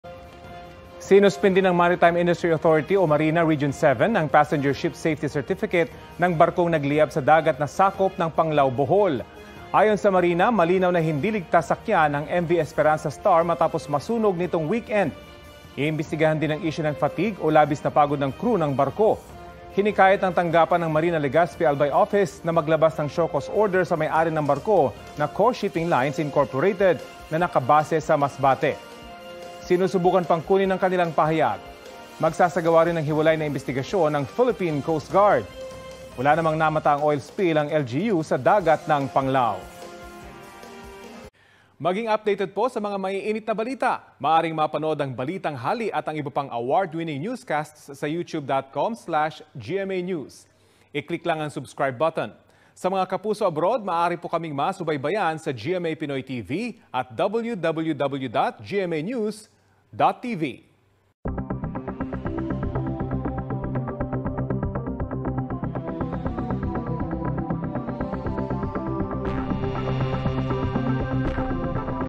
Passenger Ship Safety Certificate ng nasunog na M/V Esperanza Star, sinuspinde ng MARINA | BT
Sinuspinde ng Maritime Industry Authority o MARINA Region 7 ang Passenger Ship Safety Certificate ng barkong nagliyab sa dagat na sakop ng Panglao, Bohol.